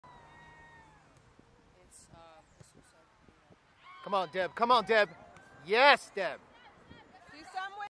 Game 15 GSA ECNL U16 vs NTH Tophat ECNL U16 001 3 clip
Deborah settles another high ball in the air, but the defender catches the settle between her knees.

Game #55537
GSA ECNL U16 vs NTH Tophat ECNL U16
GSA, Spencer, 11AM